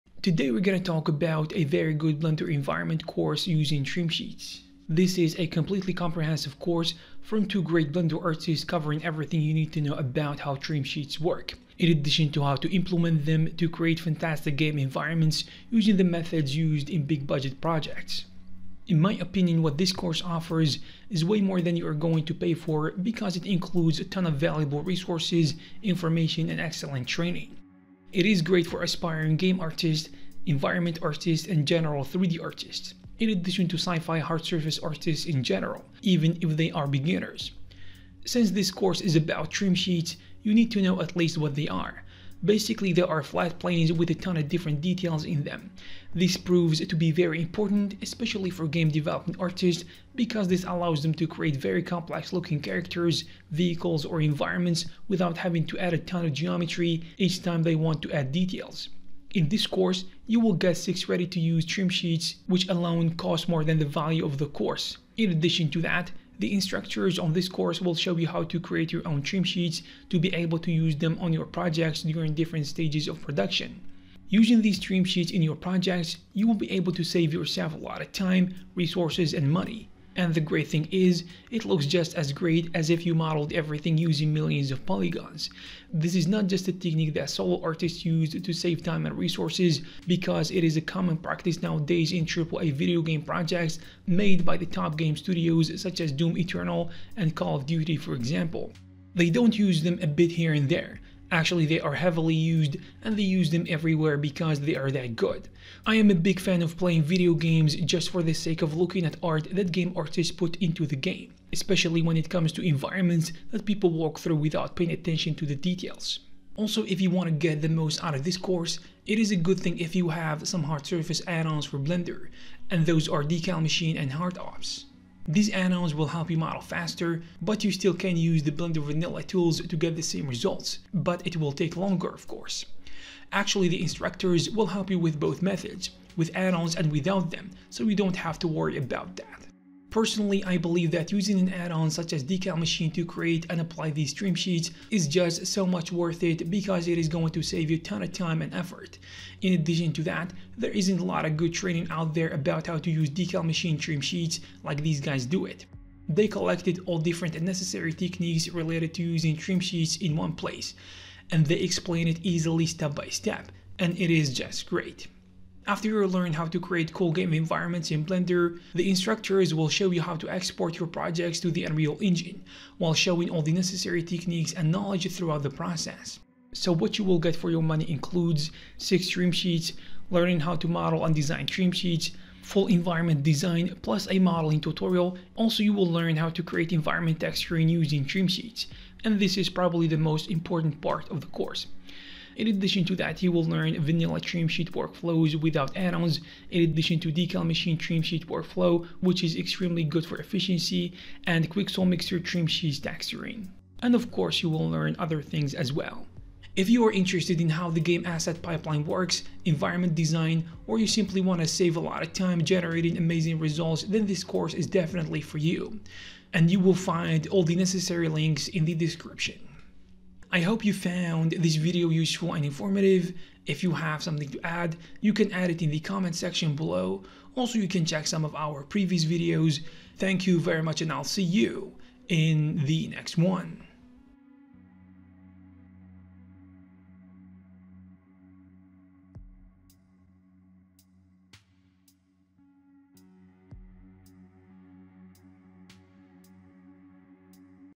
Environment Design in Blender Using Trim Sheets
Today we are going to talk about a very good Blender environment creation course using trim sheets.

This is a completely comprehensive course from Two great Blender artists covering everything you need to know about how Trim Sheets work. In addition to how you can implement them to create fantastic game environments using the methods used in big-budget game projects.